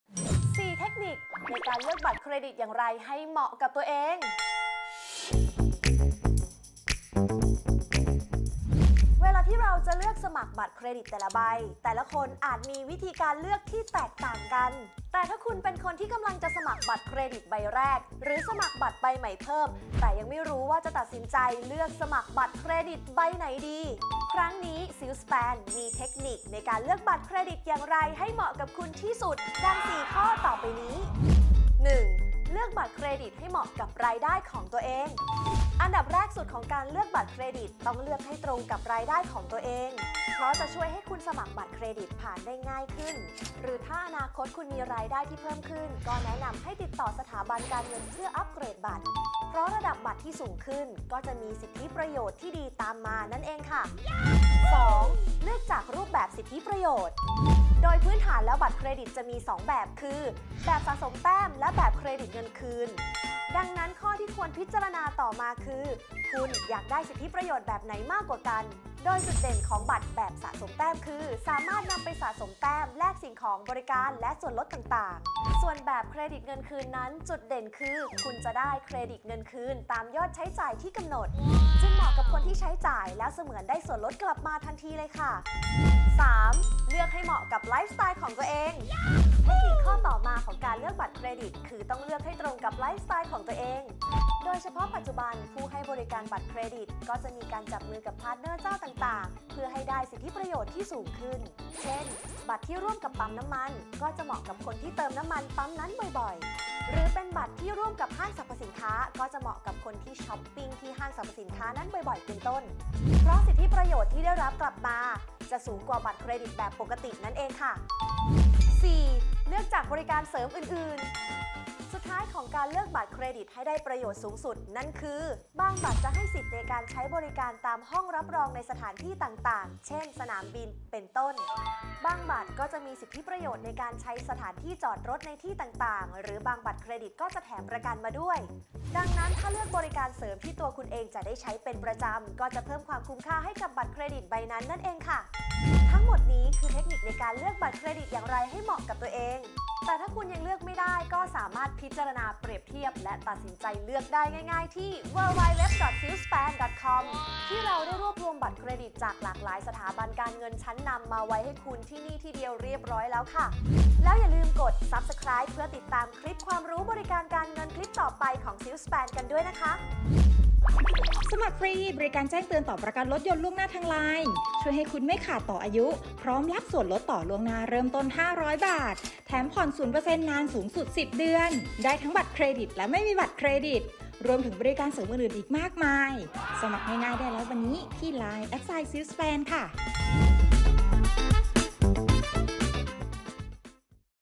4 เทคนิคเลือกบัตรเครดิตให้เหมาะกับตัวเอง | SILKSPAN
4 เทคนิคเลือกบัตรเครดิตให้เหมาะกับตัวเอง | SILKSPAN

ไม่ว่าคุณจะเป็นคนที่กำลังจะมีบัตรเครดิตใบแรก หรือมีบัตรเครดิตมาแล้วหลายใบ แต่ถ้ากำลังลังเลใจอยู่ว่าจะทำบัตรเครดิตอะไรดี ครั้งนี้ SILKSPAN มีเทคนิคในการพิจารณาเลือกบัตรเครดิตที่เหมาะสมกับคุณมาฝากกันค่ะ

มาเปรียบเทียบบัตรเครดิตจากหลากหลายสถาบันการเงินชั้น ที่ SILKSPAN รวบรวมมาให้เปรียบเทียบ และสมัครง่ายๆ ได้ที่

Please watch: "5 ข้อห้าม ไม่อยากให้แอร์รถไม่เย็น แอร์พังช่วงหน้าร้อนห้ามทำแบบนี้!!!"